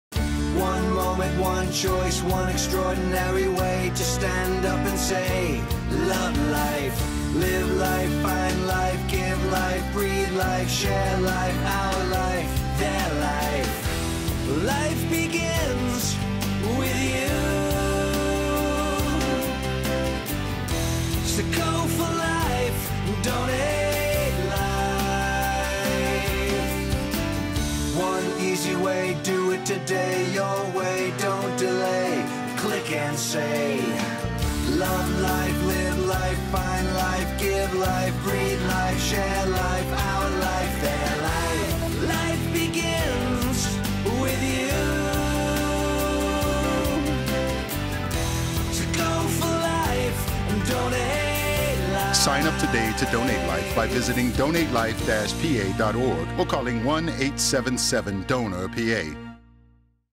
PA Organ Donor: Life Begins With You
This is the main commercial in the Donate Life PA campaign "Life Begins With You" that I developed and which ran for three years, increasing donor awareness and commitments throughout Pennsylvania. Featuring an original song written by Bryan Ward and Gary Kalinosky, produced by Rob Deaner at Market Street Sound.